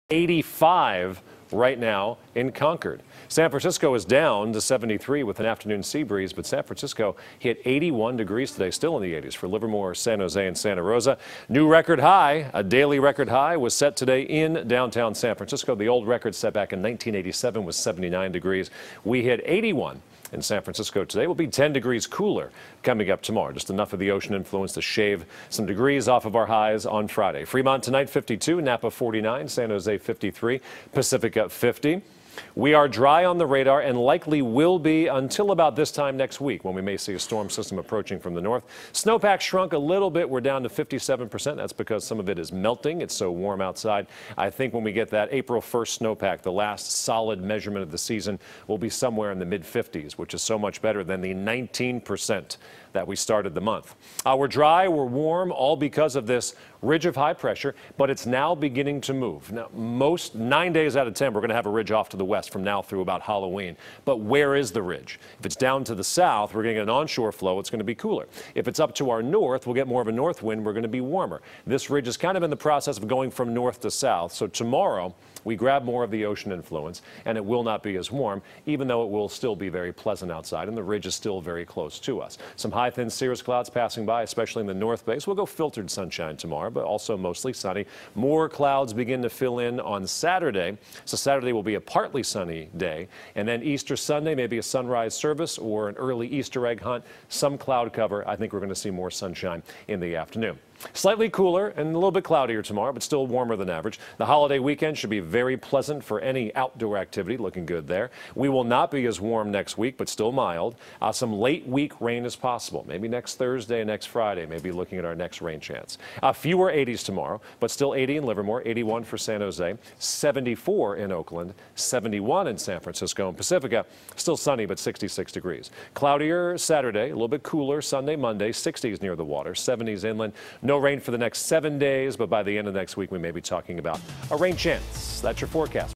Thursday Evening Pinpoint Forecast
Paul Deanno has the Thursday night forecast. (3-29-18)